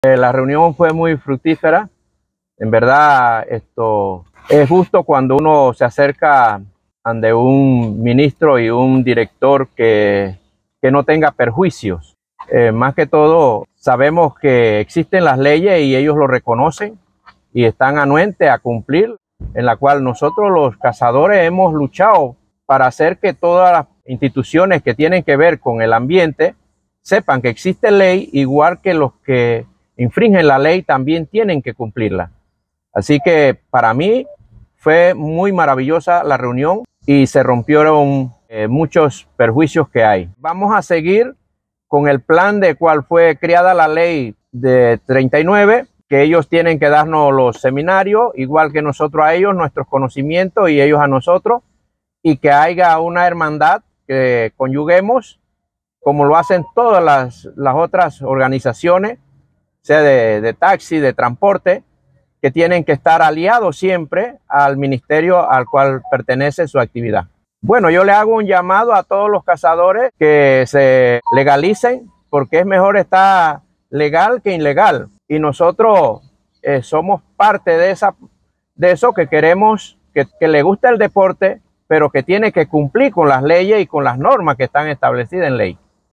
CAZADORES Y MINISTRO DE AMBIENTE SE REUNEN
Para poder ganar más y mejores logros en favor de la cacería, debemos trabajar en conjunto con las autoridades y hoy tuvimos una apertura que desde hace mucho tiempo no se nos permitía.

En una reunión en donde no se nos cuestionó nuestro propósito de cazadores, sino que se nos abrió un espacio en donde el ministro Milciades Concepción escucho nuestras quejas y en favor de las Leyes dio órdenes de ejecutar medidas y acciones que benefician a la población en general para educar desde la Fuerza Pública hasta a los campesinos de todo el país.

La educación es la mejor herramienta para poder evitar que más personas sean cazadores furtivos, que menos familias sufran por sus parientes, víctimas del abandono de las autoridades, cayendo detenidos por cometer delitos de caza, que por muchos años fueron su único medio de subsistir.

El momento es ahora y ya comenzamos, el objetivo es que la educación llegue a todos los lugares del país para que seamos un solo equipo de cazalegalpanama